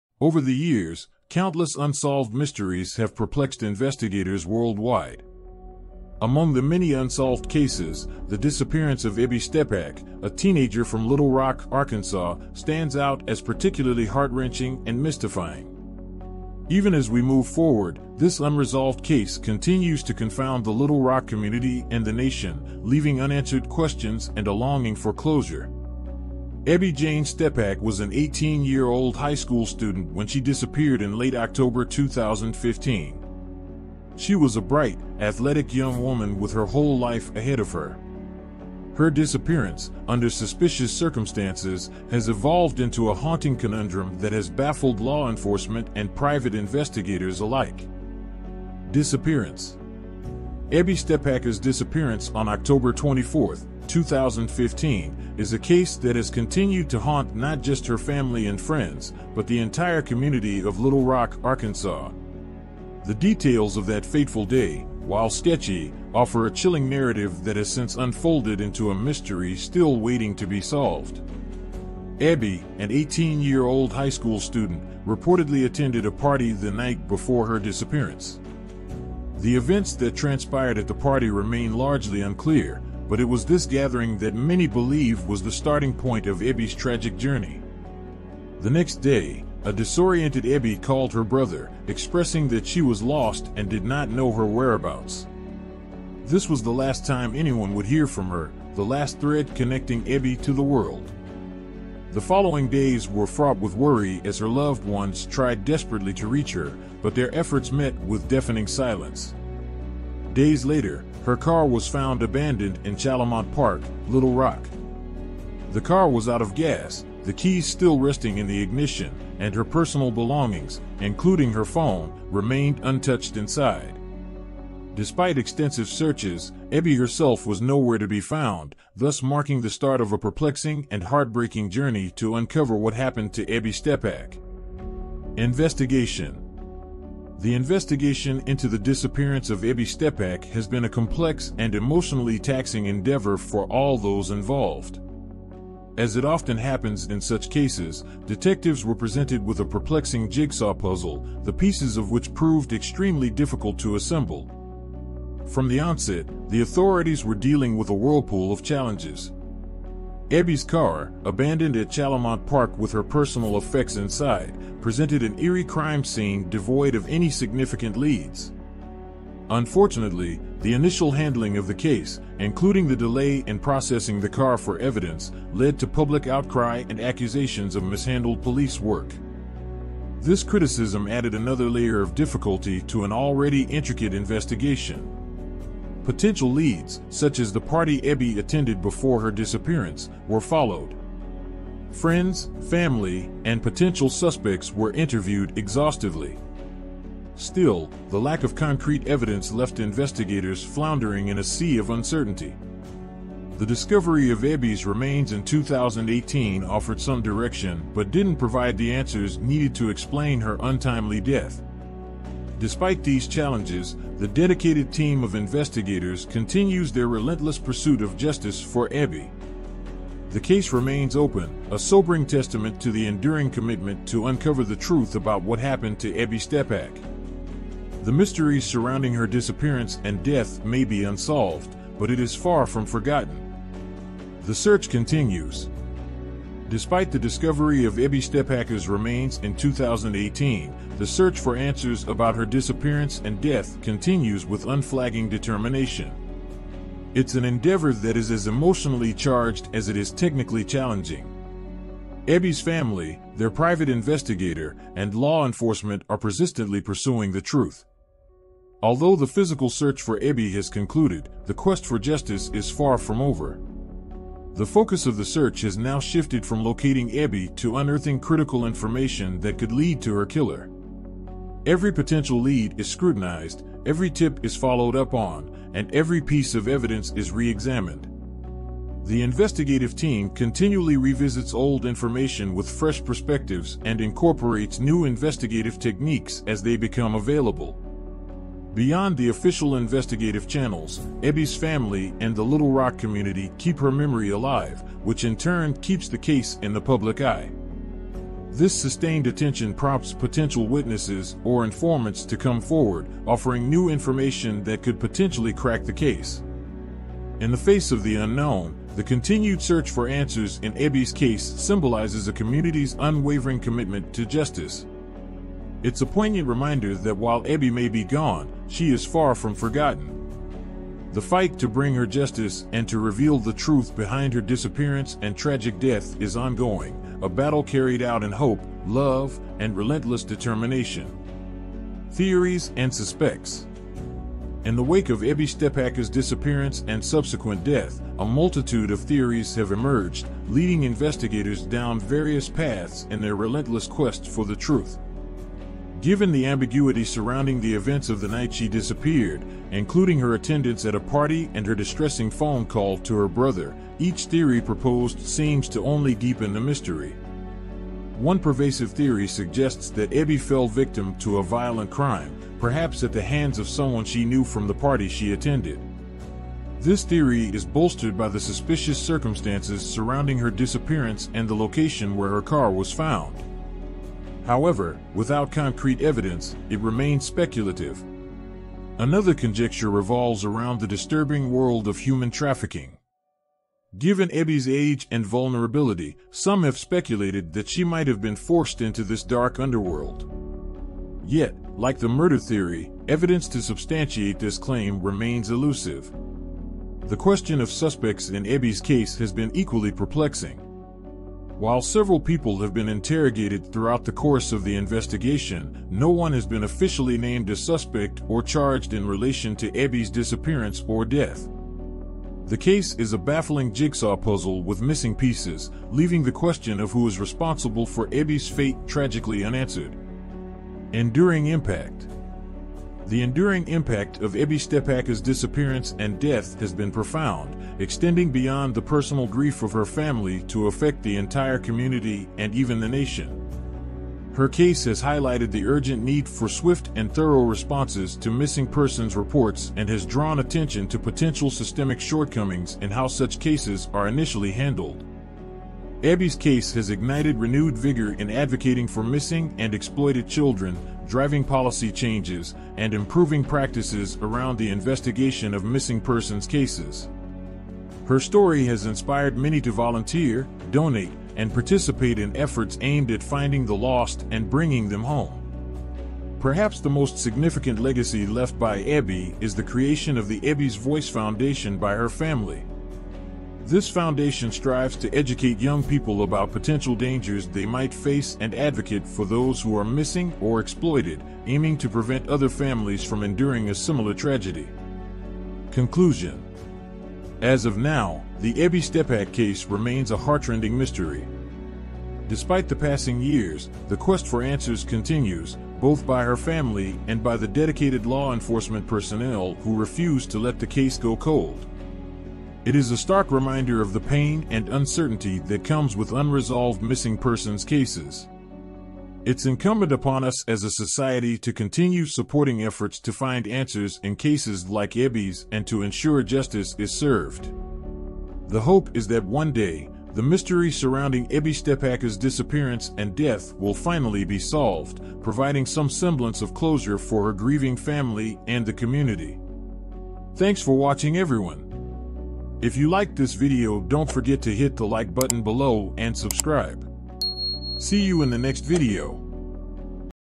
Vanished in Little Rock | The Unbreakable Enigma of Ebby Steppach's Tragic Mystery
In this meticulously crafted and deeply investigative documentary, we embark on an emotional journey into the perplexing and tragic case of Ebby Steppach, an 18-year-old girl who vanished without a trace in Little Rock, Arkansas, back in 2015. The enigma surrounding her disappearance has left her loved ones and the community haunted by unanswered questions for years.

Through extensive interviews with family members, law enforcement personnel, and key individuals closely tied to the case, we piece together a compelling and comprehensive narrative that leaves no stone unturned. This powerful video provides an unfiltered and unflinching look into every aspect of the investigation, delving into the haunting details and complexities of this heart-wrenching mystery.

As we explore the depths of this tragic event, we'll uncover the myriad of theories that have emerged over time, each offering potential insight into what might have befallen Ebby that fateful day. The emotional impact of her disappearance is profound, and we witness the indomitable spirit of the community of Little Rock as they persistently seek answers and justice.

Our aim is to bring awareness to Ebby's story and amplify the voices of those tirelessly working to solve the puzzle surrounding her vanishing. With a focus on empathy and understanding, we examine the circumstances leading up to her disappearance, scrutinizing the available evidence and shedding light on the ongoing efforts to piece together the truth.

Join us on this quest for truth and justice, as we shine a light on the shadows that have lingered for far too long. Together, let's honor Ebby Steppach's memory and support the relentless pursuit of resolution in a case that continues to leave a lasting impact on the hearts and minds of all those touched by this tragic mystery.